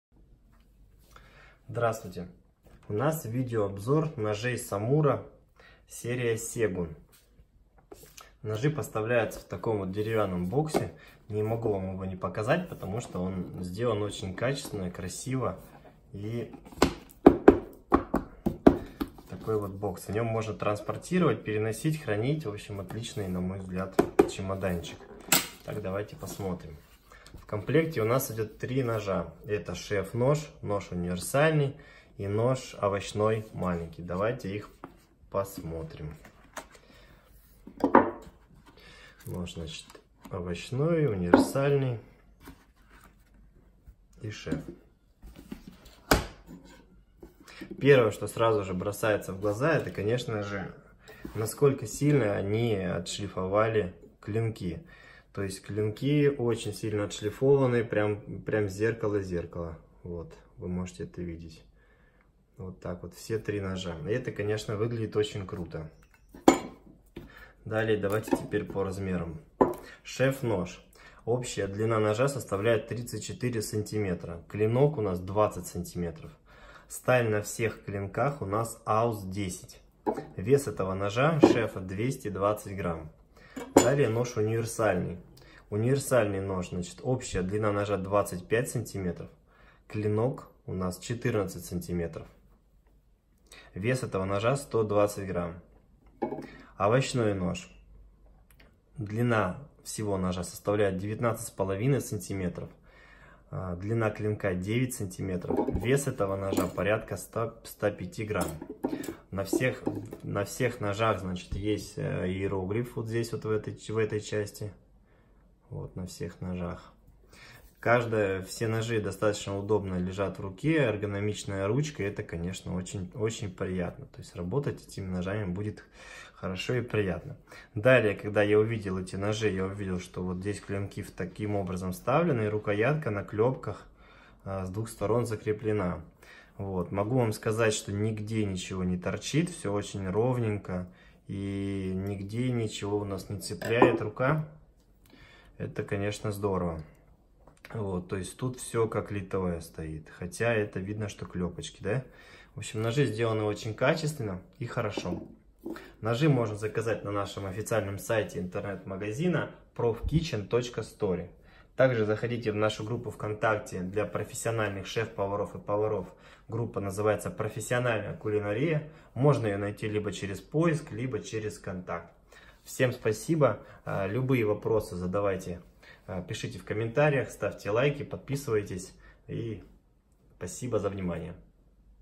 SAMURA SEGUN. НАБОР ИЗ 3-Х НОЖЕЙ
Набор из 3 ножей Samura Segun
Сталь на клинках: AUS 10

Шеф нож:
Длина  клинка: 20 см
Длина ножа: 34 см
Вес ножа: 220 гр

Нож универсальный:
Длина  клинка: 14 см
Длина ножа: 25 см
Вес ножа: 120 гр

Нож овощной:
Длина  клинка: 9 см
Длина ножа: 19,5 см
Вес ножа: 105 см